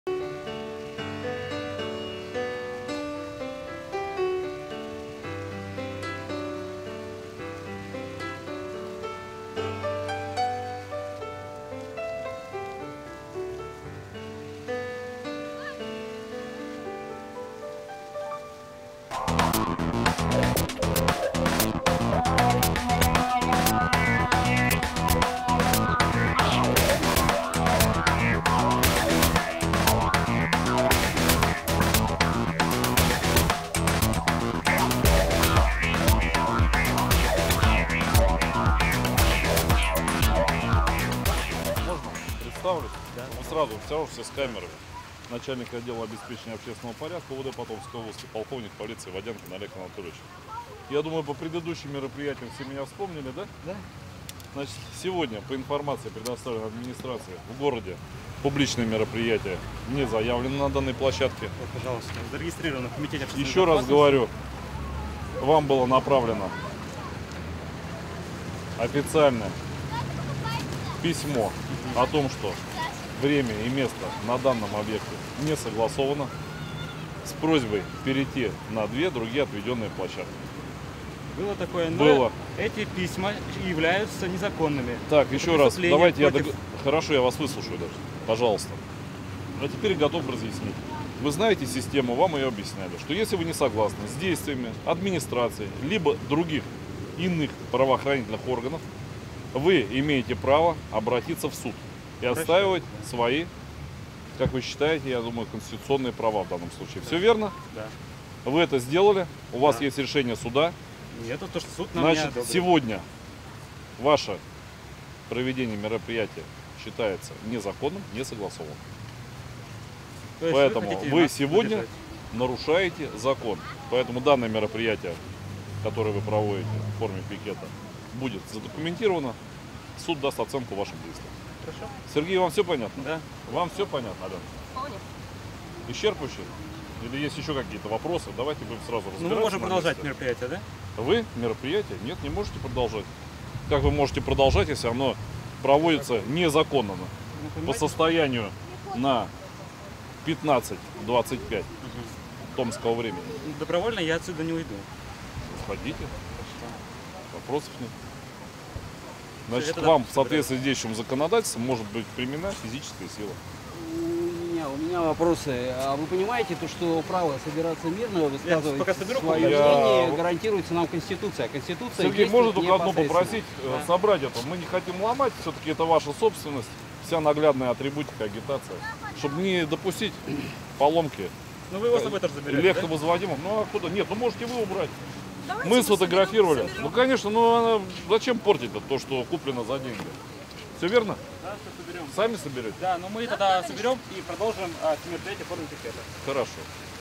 Спецоперация по захвату волонтеров штаба Навального
Полиция задержала четырех волонтеров томского штаба Алексея Навального. Подробнее —